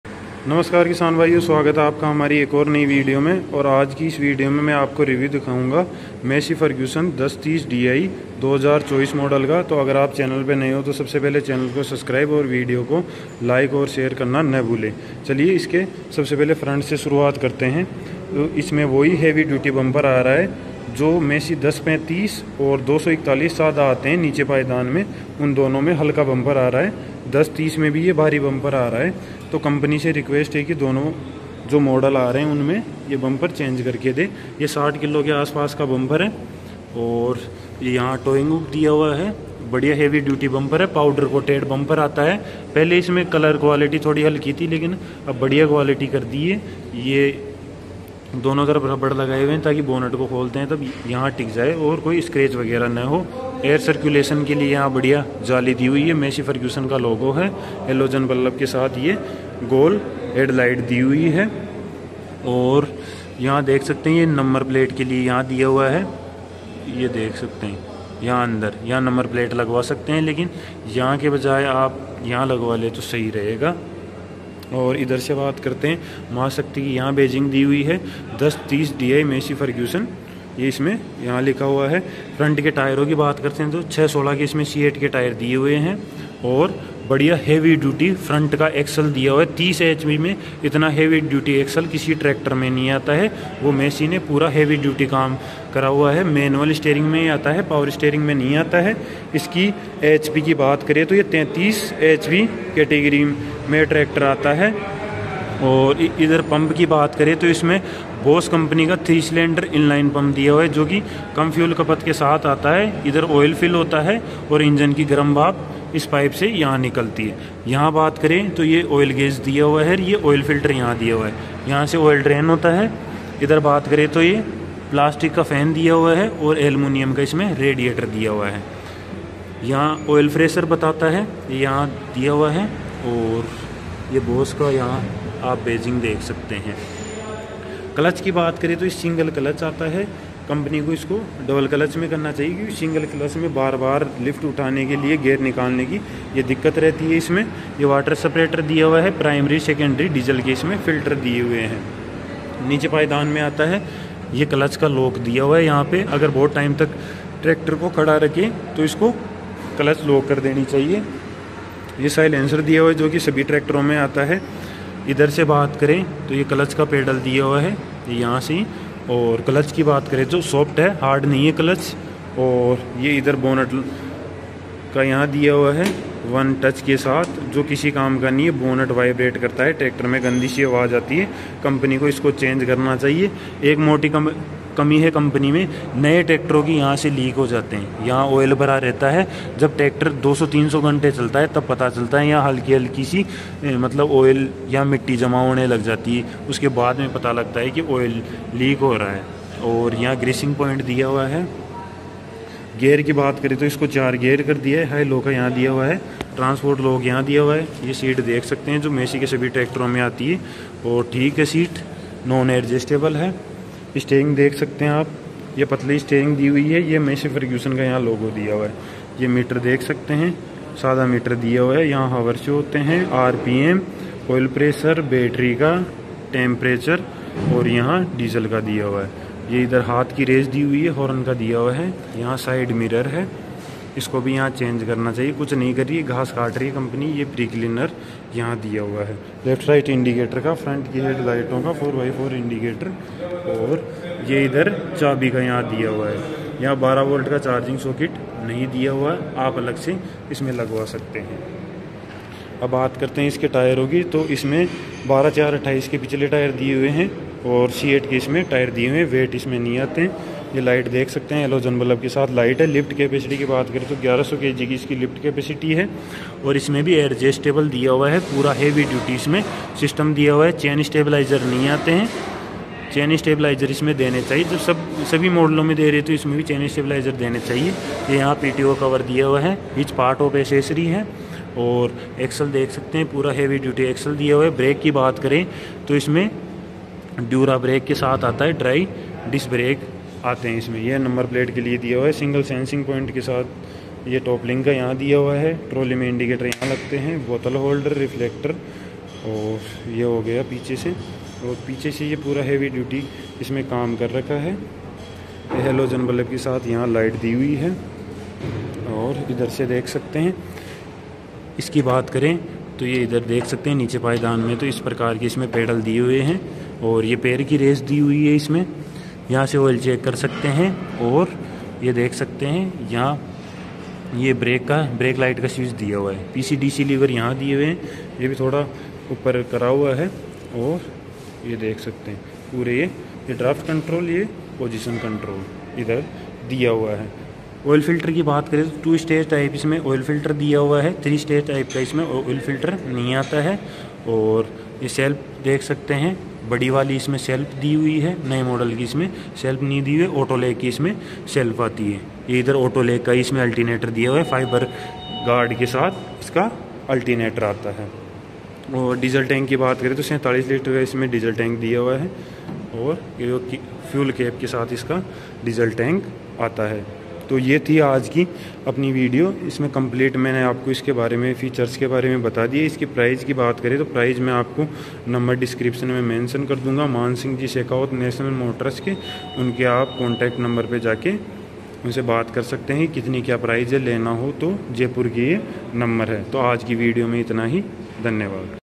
Massey Ferguson 1030 Di 33HP Review 2024 | भारत का सबसे ज़्यादा माइलेज देने वाला ट्रैक्टर
नया ट्रैक्टर खरीदने के लिए सम्पर्क करे 👇🏻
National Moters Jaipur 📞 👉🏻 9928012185